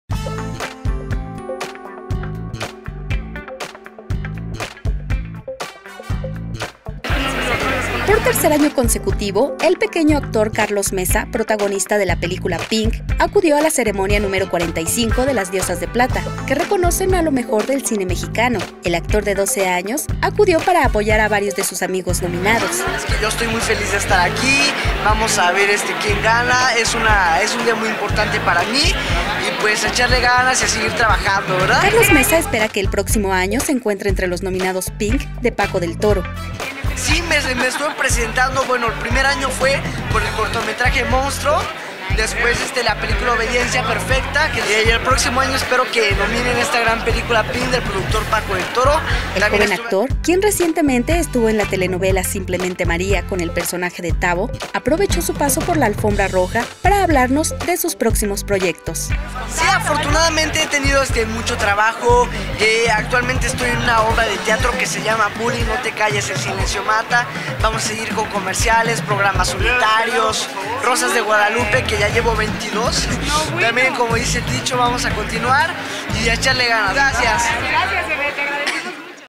Entrevista con Carlos Meza
El joven actor Carlos Meza nos cuenta de sus proyectas y como ha sido el rápido ascenso en su apenas naciente carrera.